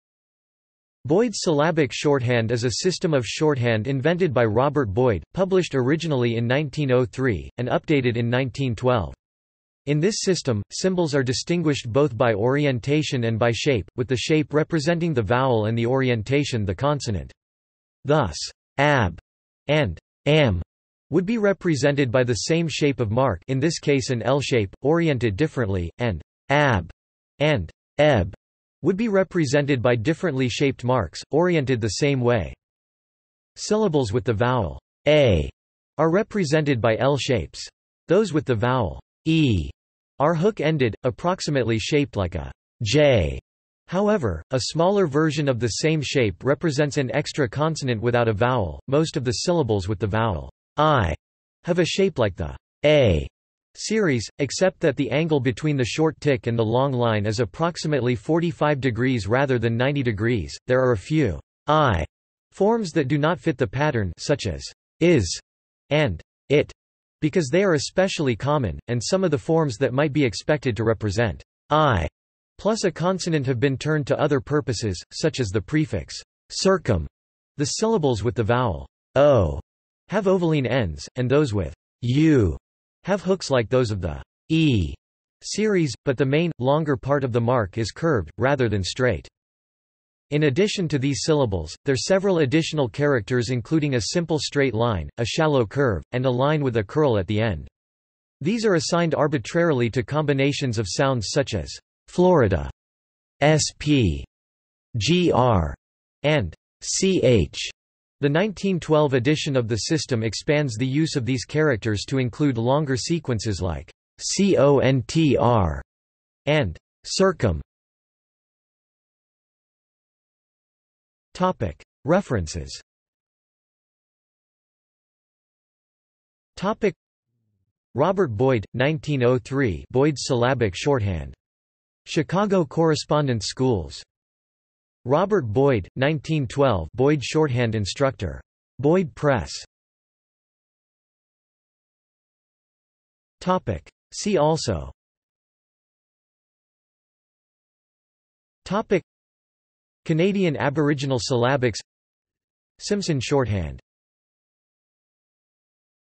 Boyd's syllabic shorthand | Wikipedia audio article
This is an audio version of the Wikipedia Article:
Boyd's syllabic shorthand

SUMMARY
Boyd's syllabic shorthand is a system of shorthand invented by Robert Boyd, published originally in 1903, and updated in 1912. In this system, symbols are distinguished both by orientation and by shape, with the shape representing the vowel and the orientation the consonant. Thus, "ab" and "am" would be represented by the same shape of mark (in this case an L-shape), oriented differently; and "ab" and "eb" would be represented by differently shaped marks, oriented the same way.
Syllables with the vowel "a" are represented by L shapes. Those with the vowel "e" are hook-ended, approximately shaped like a "J." (However, a smaller version of the same shape represents an extra consonant without a vowel.) Most of the syllables with the vowel "i" have a shape like the "a" series, except that the angle between the short tick and the long line is approximately 45° rather than 90°; there are a few "i" forms that do not fit the pattern (such as "is" and "it") because they are especially common, and some of the forms that might be expected to represent "i" plus a consonant have been turned to other purposes, such as the prefix "circum-." The syllables with the vowel "o" have ovaline ends, and those with "u" have hooks like those of the "e" series, but the main, longer part of the mark is curved, rather than straight.
In addition to these syllables, there several additional characters including a simple straight line, a shallow curve, and a line with a curl at the end. These are assigned arbitrarily to combinations of sounds such as "fl," "sp," "gr," and "ch." The 1912 edition of the system expands the use of these characters to include longer sequences like "contr" and "circum."